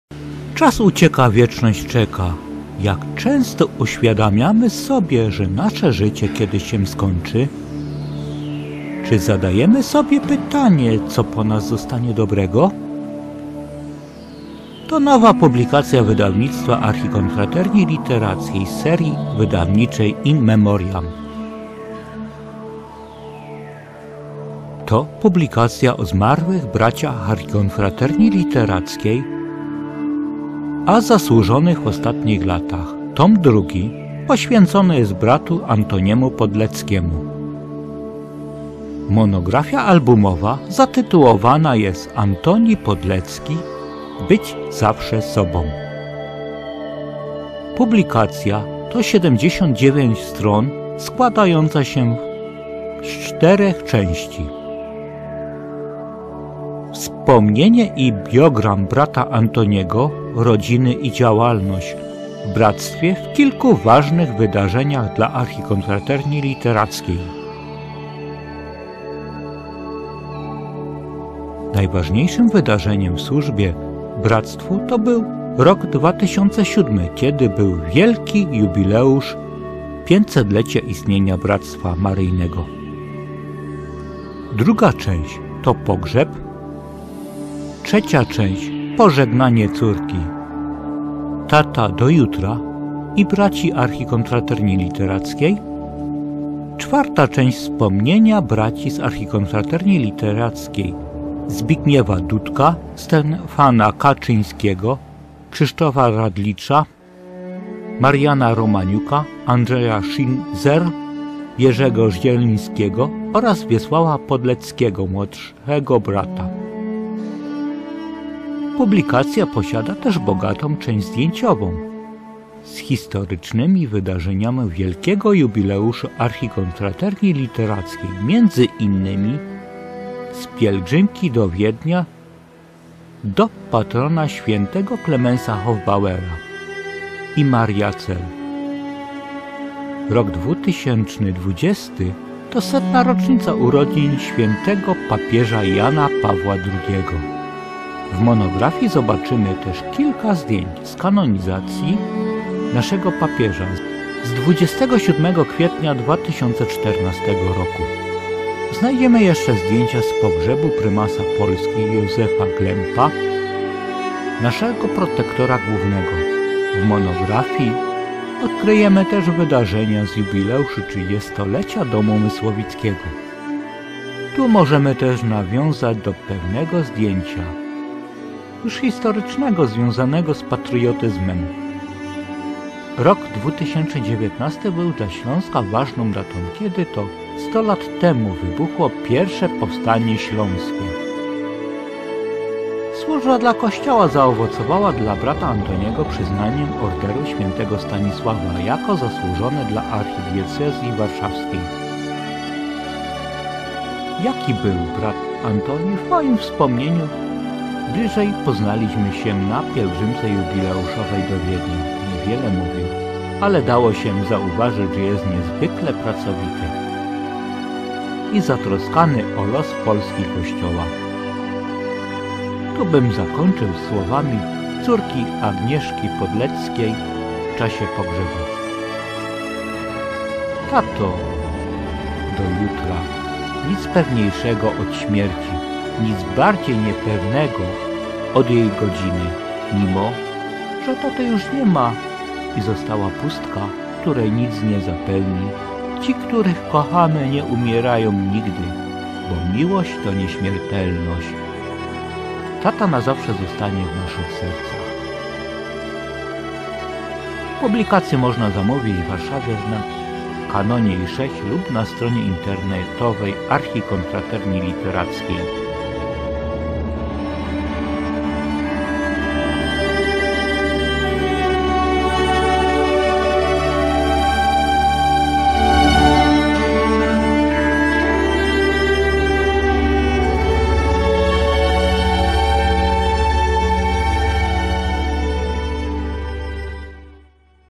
Być zawsze sobą ! Antoni Podlecki wydawnictwo Archikonfraterni Literackiej
Nowa publikacja Wydawnictwa Archikonfraterni Literackiej z serii IN MEMORIAm.
ANTONI PODLECKI (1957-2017) BYĆ ZAWSZE SOBĄ!

 Czas ucieka wieczność czeka jak często uświadamiamy sobie że nasze życie kiedyś się skończy. Czy zadajemy sobie pytanie co po nas zostanie dobrego?
To nowa publikacja wydawnictwa Archikonfraterni Literackiej.   Z serii wydawniczej IN MEMORIAm.   To publikacja o zmarłych braciach Archikonfraterni Literackiej, a zasłużonych w ostatnich latach.
Tom drugi poświecony jest bratu Antoniemu Podleckiemu.
 Monografia albumowa   zatytułowana jest:
„Antoni Podlecki – być zawsze sobą!”.
 Publikacja to 79 stron składająca się z czterech części:
• Wspomnienie i biogram brata Antoniego rodziny i działalności w bractwie w kilku ważnych wydarzeniach dla Archikonfraterni Literackiej,   Najważniejszym wydarzeniem w służbie bractwu to był rok 2007, kiedy był wielki jubileusz 500 - lecia istnienia bractwa Maryjnego.
• Druga część- to pogrzeb
• Trzecia część pożegnanie córki Tato do jutra ! i Braci z Archikonfraterni Literackiej
• Czwarta część wspomnienia braci z Archikonfraterni Literackiej : Zbigniewa Dudka, Stefana Kaczyńskiego, Krzysztofa Radlicza Mariana Romaniuka, Andrzeja Schinzel, Jerzego Zielińskiego, oraz Wiesława Podleckiego młodszego brata.
Publikacja posiada też bogatą część zdjęciową z historycznych wydarzeń wielkiego jubileuszu Archikonfraterni Literackiej między innymi z pielgrzymki do Wiednia do patrona świętego Klemensa Hofbauera i Mariazell. Rok 2020 to setna rocznica urodzin świętego Papieża Jana Pawła II. W monografii zobaczymy też kilka zdjęć z kanonizacji naszego Papieża z 27 kwietnia 2014. Znajdziemy jeszcze zdjęcia z pogrzebu prymasa Polski Józefa Glempa naszego protektora głównego. W monografii odkryjemy też wydarzenia z jubileuszu  30-lecia domu Mysłowickiego. Tu możemy też nawiązać do pewnego zdjęcia już historycznego związanego z patriotyzmem. Rok 2019 był dla Śląska ważną datą kiedy to 100 lat temu wybuchło pierwsze powstanie Śląskie. Służba dla kościoła zaowocowała dla brata Antoniego przyznaniem orderu Świętego Stanisława jako zasłużony dla Archidiecezji Warszawskiej. Jaki był brat Antoni w moim wspomnieniu bliżej poznaliśmy się na pielgrzymce jubileuszowej do Wiednia niewiele mówił ale dało się zauważyć że jest niezwykle pracowity i zatroskany o los Polski i kościoła. Tu bym zakończył słowami córki Agnieszki Podleckiej w czasie pogrzebu:
 Tato! Do jutra
Nic pewniejszego od śmierci. Nic bardziej niepewnego od jej godziny. Mimo, że Taty już nie ma i została pustka, której nic nie zapełni, ci których kochamy nie umierają nigdy, bo miłość to nieśmiertelność. Tata na zawsze zostanie w naszych sercach.

Publikację można  zamówić w Warszawie na Kanoniej 6 lub na stronie internetowej Archikonfraterni Literackiej.
Jacek Źróbek brat
 Domu Mysłowickiego
Archikonfraterni Literackiej